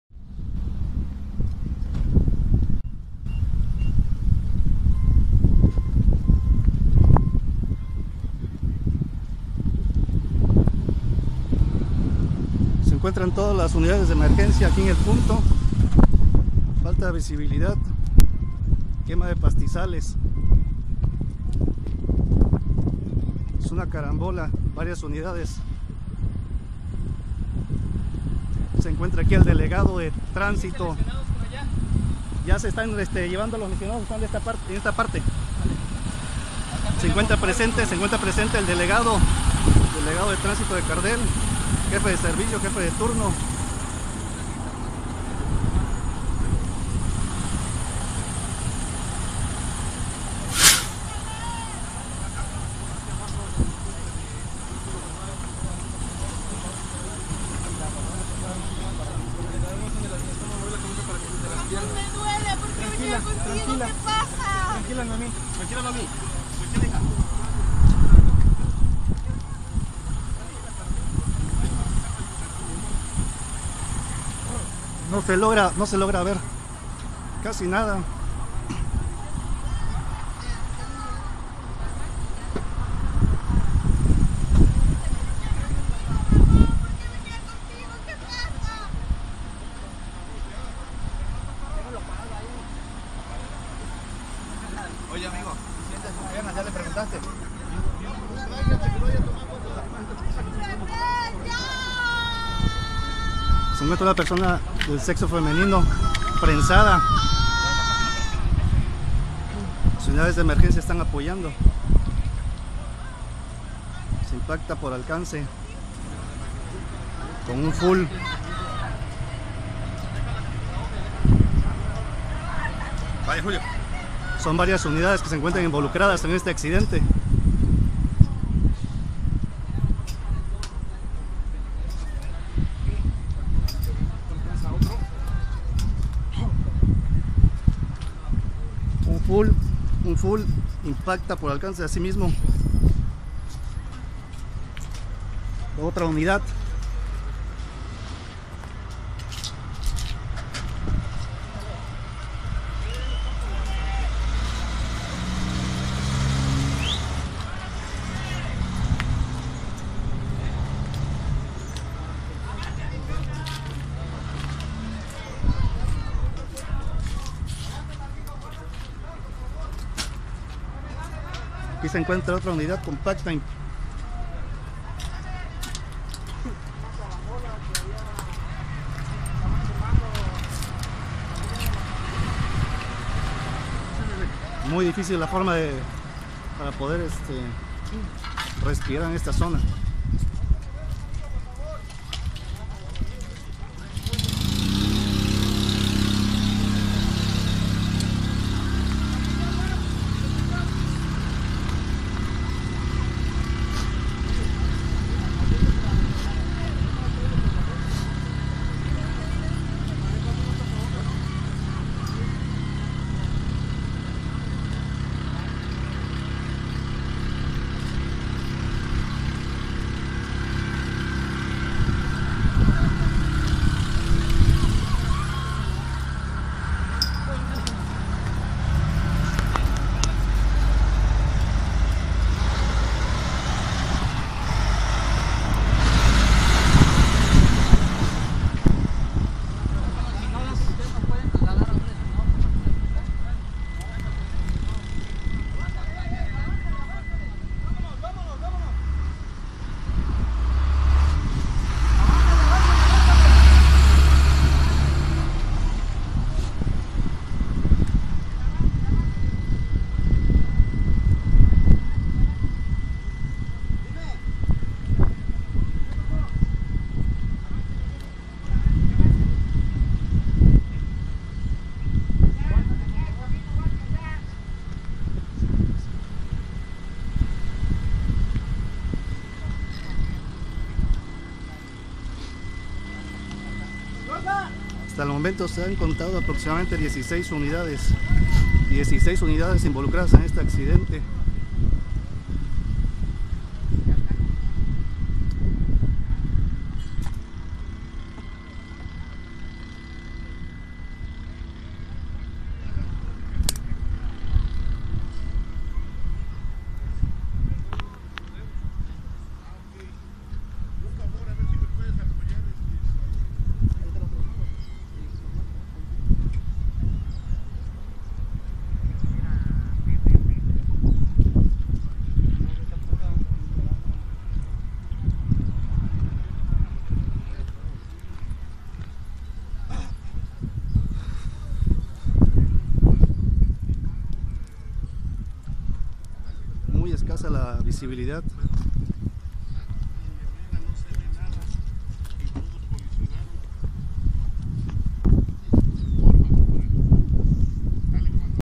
Carambola en Cardel, Veracruz
Imprudencia podría haber sido la causa de la tremenda carambola, de varios autos, el día de ayer 30 de marzo de 2018.